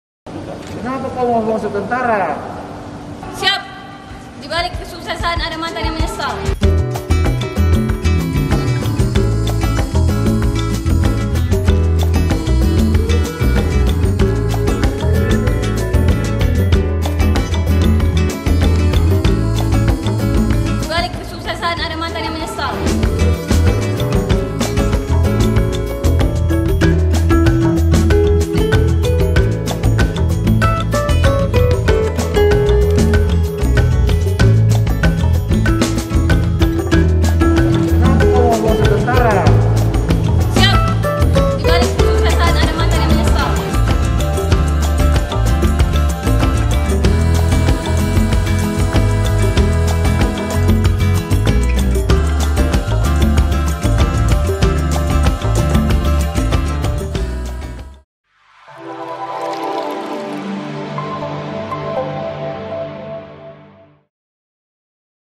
Elvina br Sembiring Viral: di Balik Kesuksesan Ada Mantan yang Menyesal 🤣
TRIBUN-MEDAN.COM - Viral video seorang calon Bintara (Kowad) TNI AD Kodam I Bukit Barisan yang memberikan jawaban lucu saat ditanya alasan mendaftar sebagai prajurit TNI.

Perempuan itu adalah Elvina br Sembiring